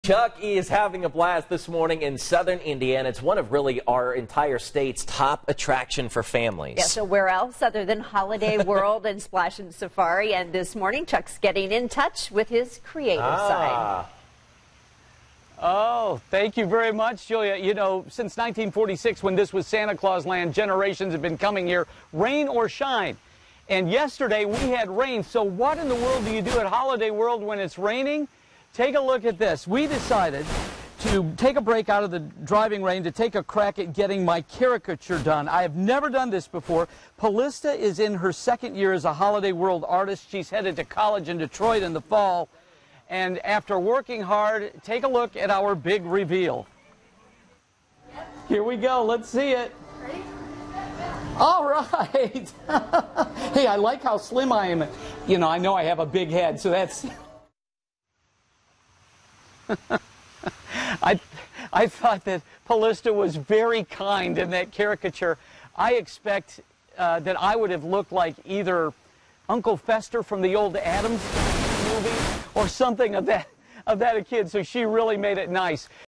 Holiday World & Splashin' Safari
There's always something fun to do at Holiday World & Splashin' Safari, even when there's rain.